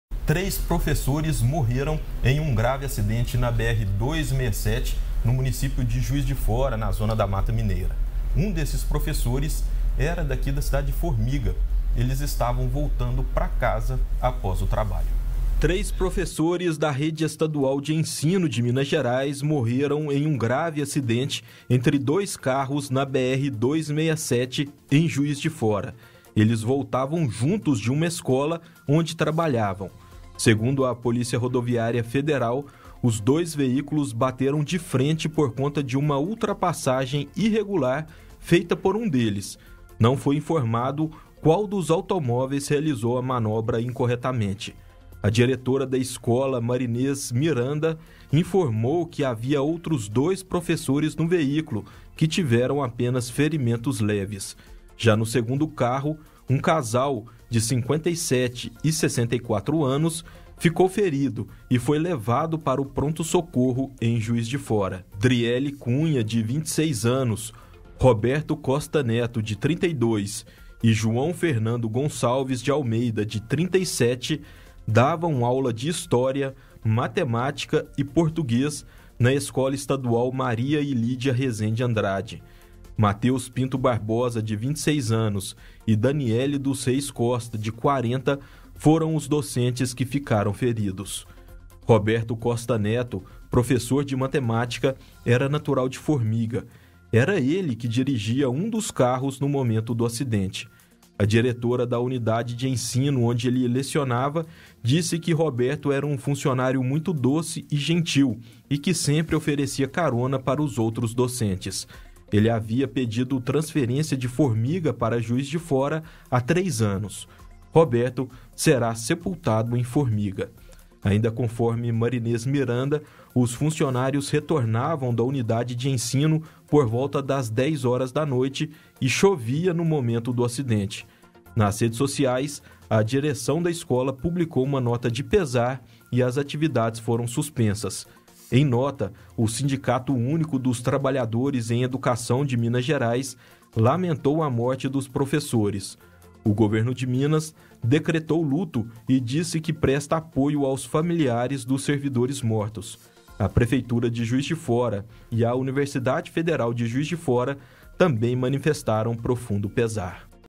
PROFESSOR DE FORMIGA MORRE EM ACIDENTE NA BR-267, EM JUIZ DE FORA
| REGIÃO |

Três professores morreram em um grave acidente na BR-267, em Juiz de Fora. Um dos professores era de Formiga. Eles estavam voltando pra casa, após o trabalho. Veja mais informações: